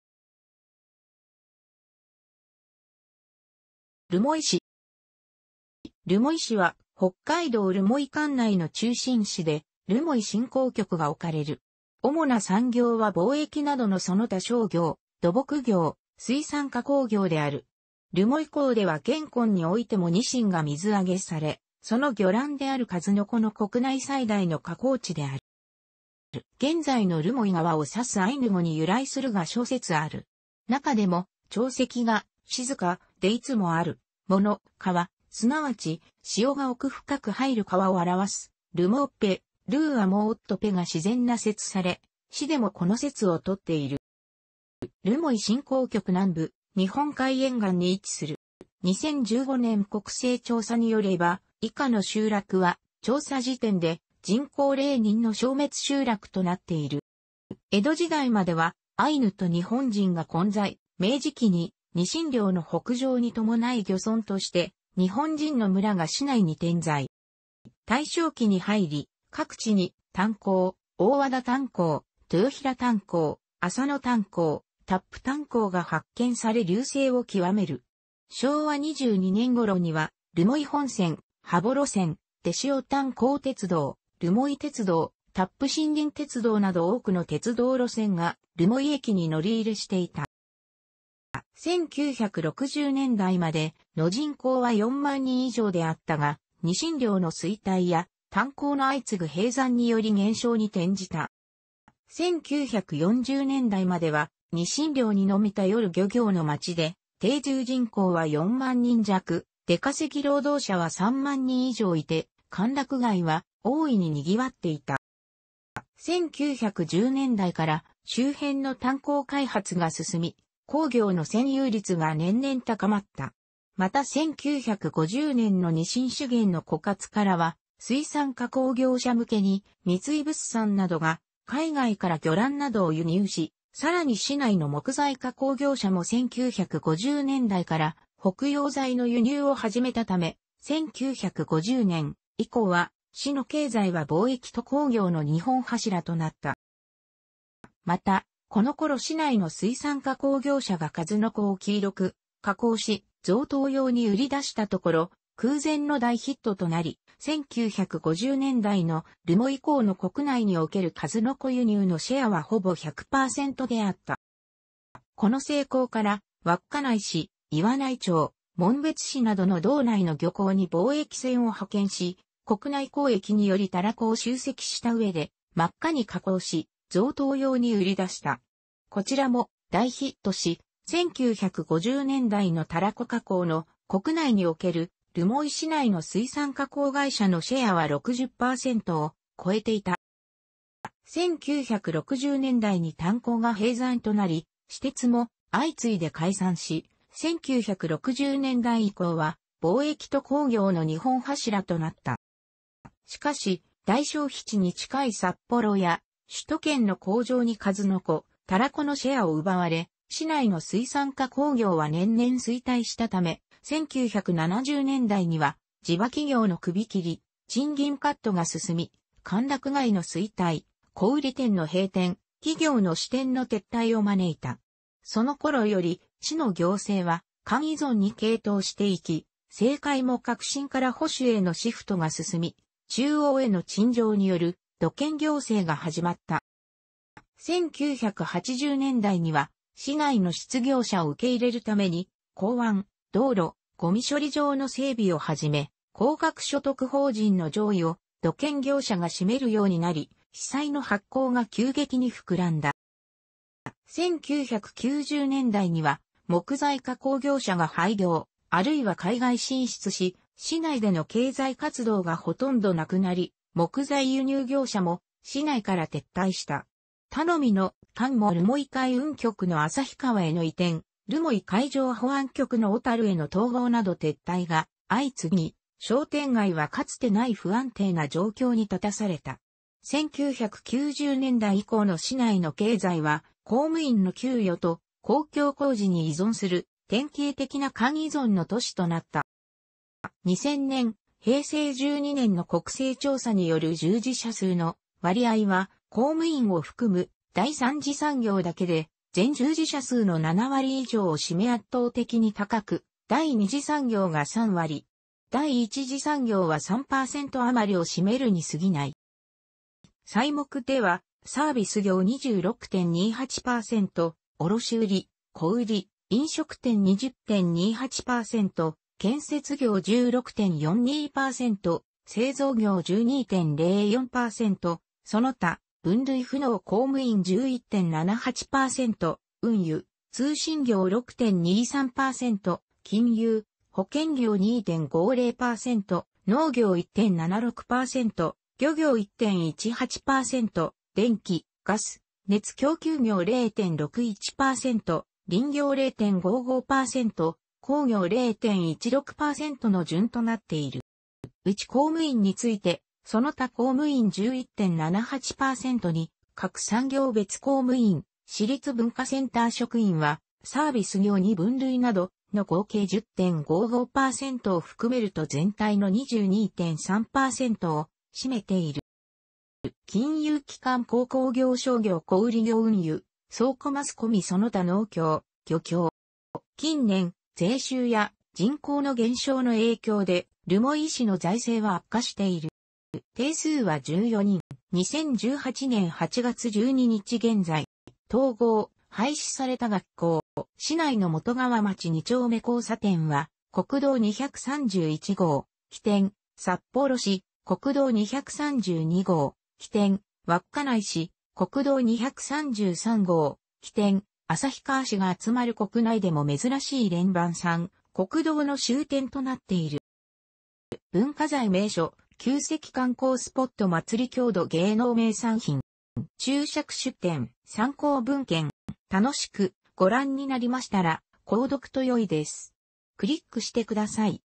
留萌市
留萌市（るもいし）は、北海道留萌管内の中心市で、留萌振興局が置かれる。主な産業は貿易などのその他商業、土木業、水産加工業である。留萌港では現今においてもニシンが水揚げされ、その魚卵であるカズノコの国内最大の加工地である。

現在の留萌川を指すアイヌ語に由来するが諸説ある。中でも「潮汐が・静か・でいつもある・もの（川）」、すなわち「汐が奥深く入る川」を表す「ルㇽモオッペ（rur-mo-ot-pe）」が自然な説とされ、市でもこの説を取っている。

留萌振興局南部、日本海沿岸に位置する。

2015年国勢調査によれば、以下の集落は調査時点で人口0人の消滅集落となっている。

江戸時代まではアイヌと日本人が混在、明治期にニシン漁の北上に伴い漁村として日本人の村が市内に点在。

大正期に入り、各地に炭鉱（大和田炭鉱、豊平炭鉱、浅野炭鉱、達布炭鉱）が発見され隆盛をきわめる。昭和22年頃には留萌本線、羽幌線、天塩炭砿鉄道、留萠鉄道、達布森林鉄道など多くの鉄道路線が留萌駅に乗り入れしていた。

1960年代までの人口は4万人以上であったが、ニシン漁の衰退や炭鉱の相次ぐ閉山により減少に転じた。

1940年代まではニシン漁にのみ頼る漁業の町で、定住人口は4万人弱、出稼ぎ労働者は3万人以上いて、歓楽街は大いに賑わっていた。

1910年代から周辺の炭鉱開発が進み、鉱業の占有率が年々高まった。また1950年のニシン資源の枯渇からは、水産加工業者向けに三井物産などが海外から魚卵などを輸入し、さらに市内の木材加工業者も1950年代から北洋材の輸入を始めたため、1950年以降は市の経済は貿易と鉱業の2本柱となった。

また、このころ市内の水産加工業者が数の子を黄色く加工し、贈答用に売り出したところ、空前の大ヒットとなり、1950年代の留萌港の国内における数の子輸入のシェアはほぼ100％であった。
この成功から稚内市、岩内町、紋別市などの道内の漁港に貿易船を派遣し、国内交易によりタラコを集積した上で、真っ赤に加工し、贈答用に売り出した。こちらも大ヒットし、1950年代のタラコ加工の国内における留萌市内の水産加工会社のシェアは60%を超えていた。

1960年代に炭鉱が閉山となり、私鉄も相次いで解散し、1960年代以降は貿易と工業の2本柱となった。

しかし、大消費地に近い札幌や首都圏の工場に数の子、タラコのシェアを奪われ、市内の水産加工業は年々衰退したため、1970年代には地場企業の首切り、賃金カットが進み、歓楽街の衰退、小売店の閉店、企業の支店の撤退を招いた。
その頃より市の行政は「官依存」に傾倒して行き、政界も革新から保守へのシフトが進み、中央への陳情による土建行政が始まった。

1980年代には市内の失業者を受け入れるために港湾、道路、ごみ処理場の整備をはじめ、高額所得法人の上位を土建業者が占めるようになり、市債の発行が急激に膨らんだ。

1990年代には木材加工業者が廃業、あるいは海外進出し、市内での経済活動がほとんどなくなり、木材輸入業者も市内から撤退した。頼みの「官」も留萌海運局の旭川への移転、留萌海上保安局の小樽への統合など撤退が相次ぎ、商店街はかつてない不安定な状況に立たされた。1990年代以降の市内の経済は公務員の給与と、公共工事に依存する典型的な官依存の都市となった。

2000年（平成12年）の国勢調査による従事者数の割合は公務員を含む第三次産業だけで全従事者数の7割以上を占め圧倒的に高く、第二次産業が3割。第1次産業は3%余りを占めるに過ぎない。

細目ではサービス業26.28%、卸売・小売・飲食店20.28%、建設業16.42%、製造業12.04%、その他（分類不能）公務員11.78%、運輸・通信業6.23%、金融・保険業2.50%、農業1.76%、漁業1.18%、電気・ガス・熱供給業0.61%、林業0.55%、工業0.16%の順となっている。

うち公務員について「その他公務員」11.78%に各産業別公務員（市立文化センター職員は「サービス業」に分類など）の合計10.55%を含めると全体の22.3%を占めている。

金融機関
鉱工業
商業
小売業
運輸・倉庫
マスコミ
その他
農協・漁協

近年、税収や人口の減少の影響で留萌市の財政は悪化している。

定数は14人。

統合・廃止された学校

市内の元川町2丁目交差点は、国道231号（ - 起点・札幌市）、国...